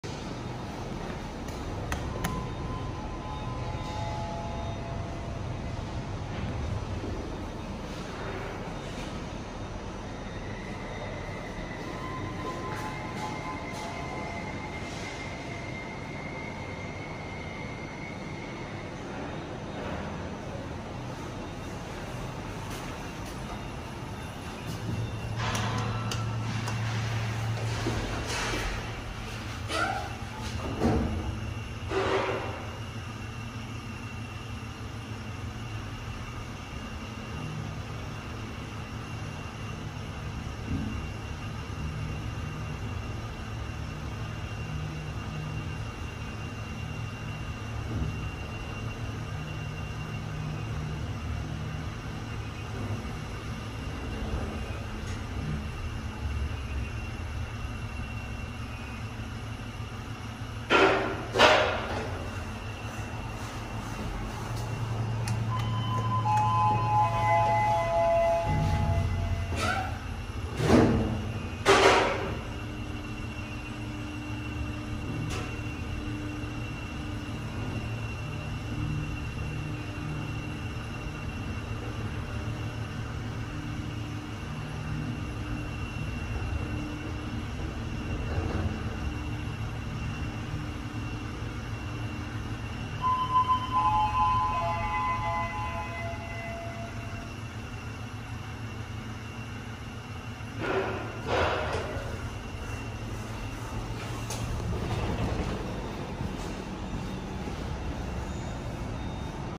TKE EVOLUTION MRL @ BLOCK 51, NGEE ANN POLYTECHNIC [FIREMAN LIFTS]
Ngee Ann Polytechnic in its Clementi campus has a wide variety of lift brands, mostly comprising of Hitachi with some being from TKE (ThyssenKrupp Elevator) and Kone.

Block 51 and Block 58 got ThyssenKrupp instead due to open contracting requirements, which meant that Hitachi may not necessarily be chosen to supply the lifts. This can also be seen in the nearby Convention Centre and Block 73, which received Kone units instead.

Block 51 has 2 fireman lifts in 2 separate lobbies on opposite ends, in order to comply with Singaporean fire codes. They serve all floors of the building as legally required. This means that they also double up as service lifts, transporting goods to areas where passenger lifts are not able to reach.